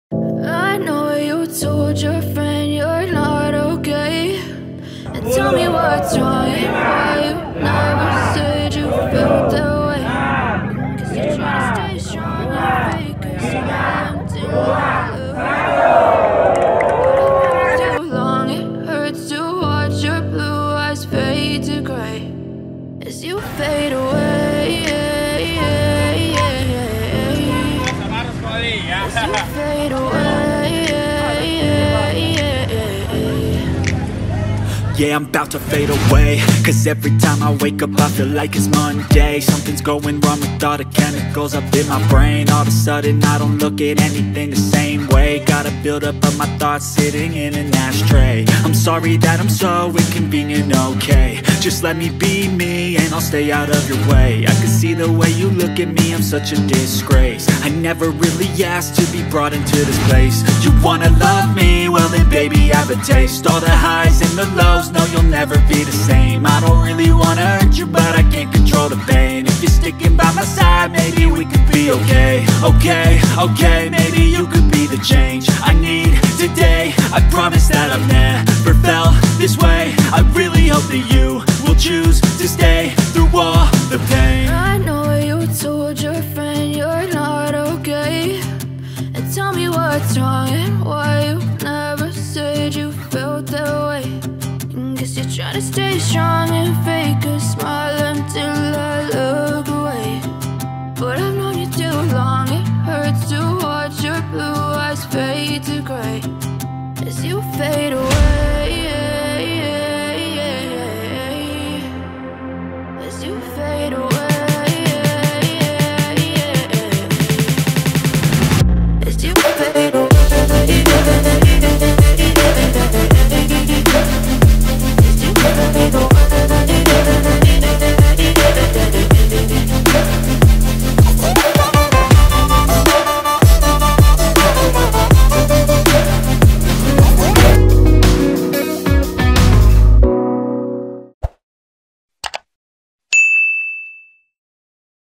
Danny Pomanto Bersama Rudianto Lallo Lepas Peserta Jalan Santai IKA SMAN 6 Makassar
Ribuan alumni SMAN 6 Makassar memeriahkan jalan santai pada puncak Milad ke-35, Minggu 29/5/2022. Adapun jalur jalan santai star di Sekolah SMA 6 Makassar Jalan Ir Sutami menuju jalan Kapasa Raya dan finish kembali di Jalan Ir Sutami.

Video terkini dan terbaru..